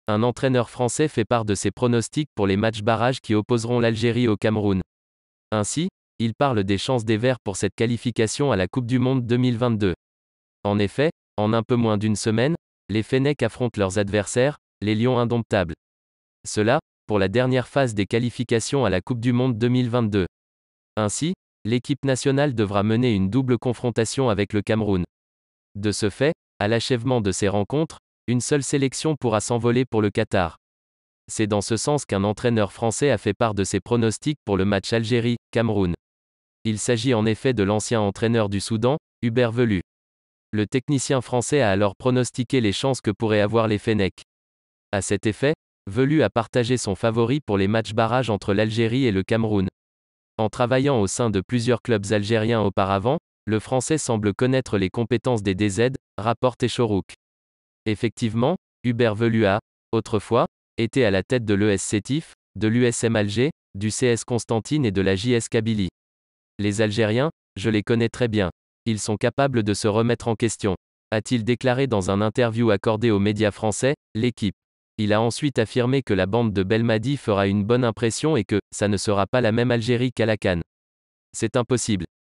Algérie – Cameroun : le pronostic favorable aux Fennecs par l'entraîneur français Hubert Velud
Un entraîneur français parle des chances des Verts pour cette qualification à la Coupe du Monde 2022, ainsi, fait part de ses pronostics pour les matchs barrages qui opposeront l’Algérie au Cameroun. En effet, à un peu moins d’une semaine de la rencontre des Fennecs avec leurs adversaires, les Lions Indomptables. L’entraîneur et technicien français, Hubert Velud à pronostiquer les chances que pourraient avoir les Fennecs. À cet effet, Velud a partagé son favori pour les matchs barrages entre l’Algérie et le Cameroun.